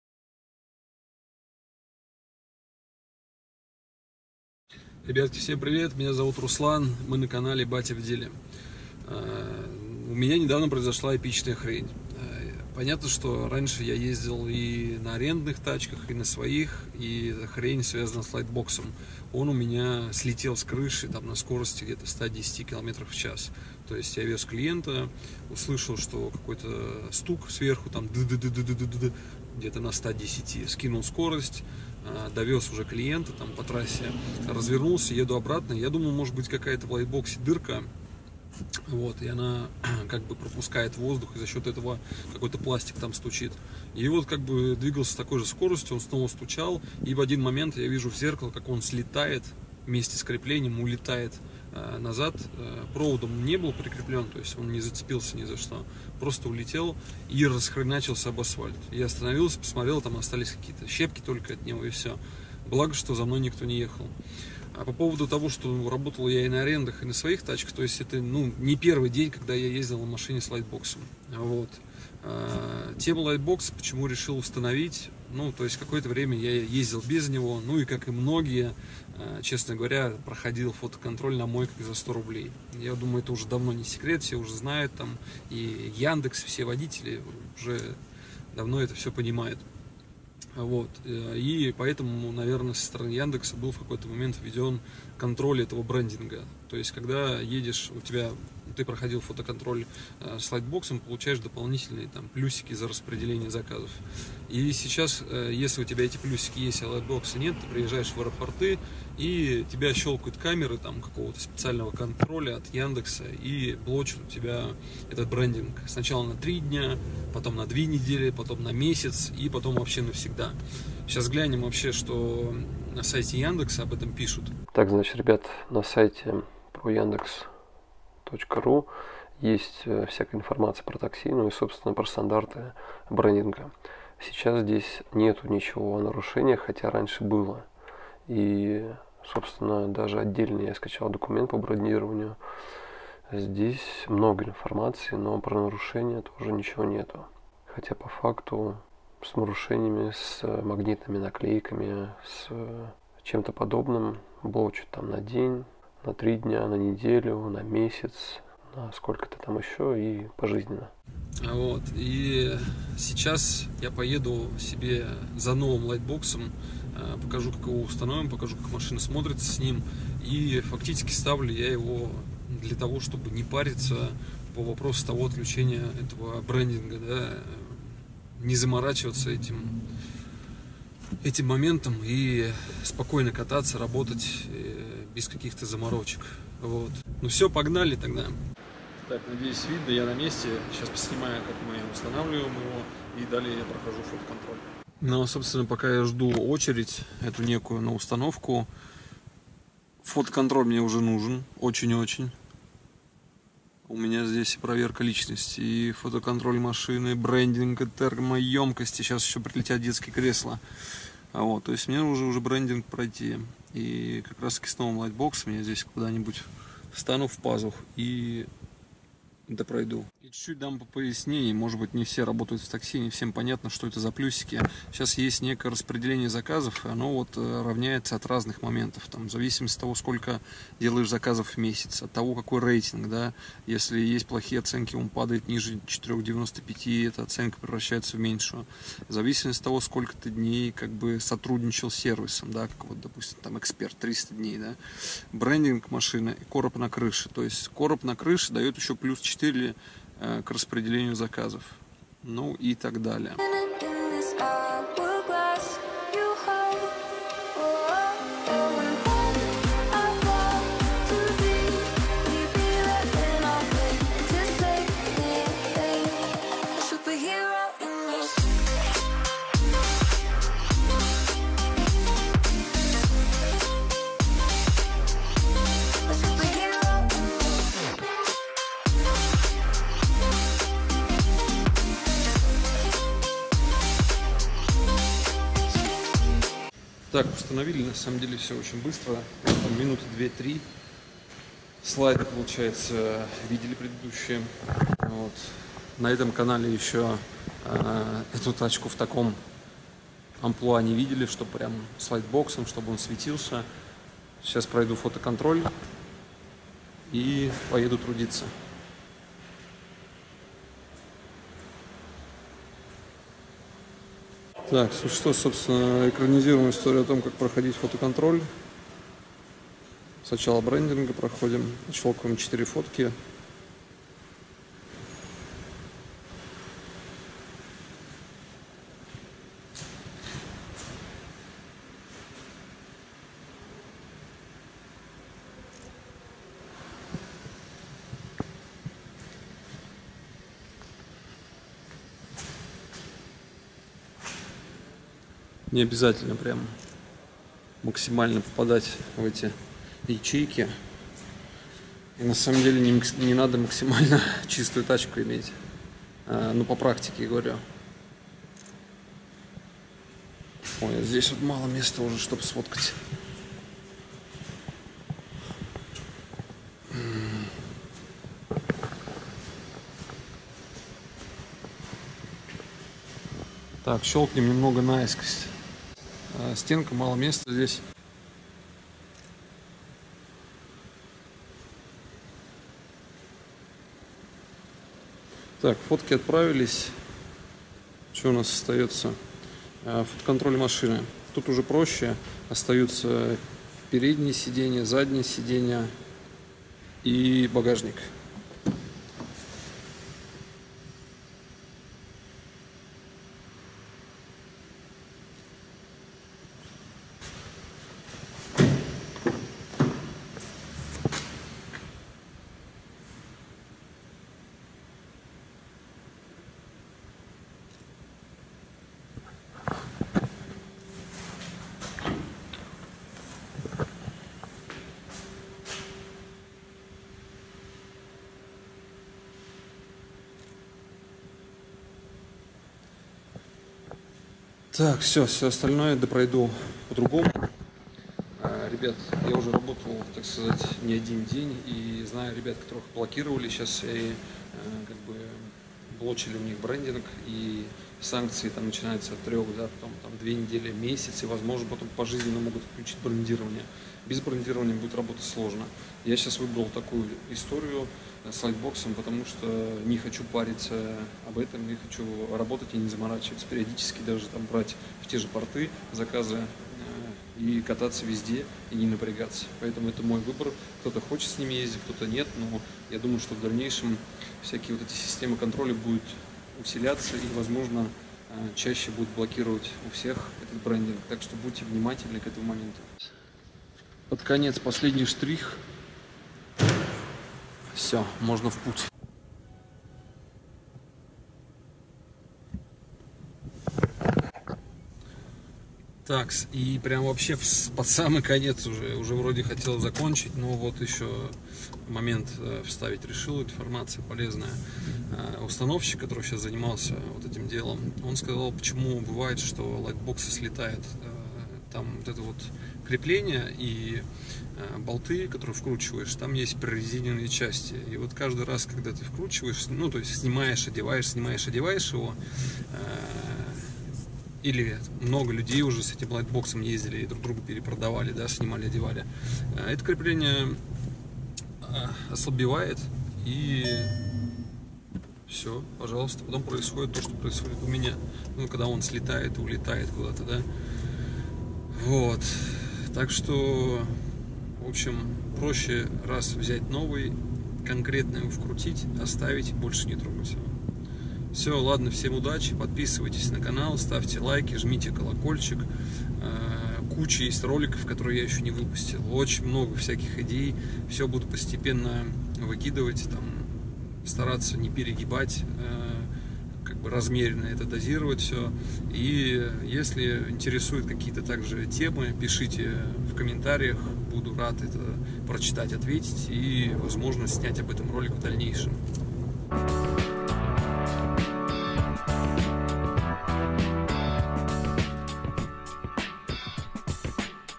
Почему слетают ЛАЙТБОКСЫ с крыши? | Брендирование ЯНДЕКС.ТАКСИ
Ребята, доброго времени суток! Однажды я вёз клиента и в моменте разогнался до 110 км по трассе, я и представить не мог, что у меня может оторвать с крыши лайтбокс. Вся эта история могла намного печальнее закончиться, благо всё обошлось и этот лайтбокс ни в кого не попал. Просто знайте, что такое возможно! Вообщем, я теперь буду понимать, что такое возможно и в ролике вы узнаете, как в целом этого можно избежать. Ставлю новый короб и стартую с ним работать. Любые ваши мысли о ролике жду ниже в комментариях.